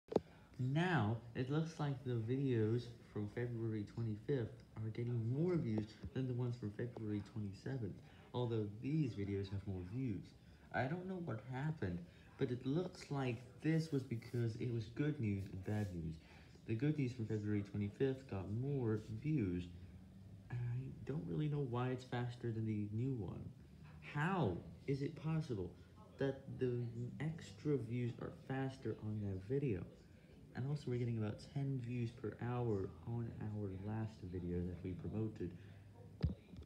The views are faster on my older videos?!
I saw faster views on my older one.